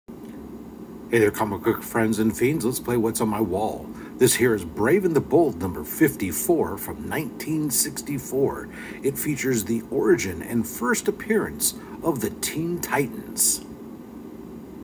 What comics are on my wall part 11
Brave and the Bold issue 54  (June 1964)
1st appearance and origin of the Teen Titans

Robin, Kid Flash and Aqualad are all invited to come to a town where the teenagers and the adults are in the middle of a dispute. When the villainous Mr. Twister shows up and captures all the town's kids, the three teen heroes formulate a plan to identify and interrupt the source of the man's power.

Writer Bob Haney
Artist Bruno Premiani
Cover Artist Bruno Premiani
Editor Murray Boltinoff | George Kashdan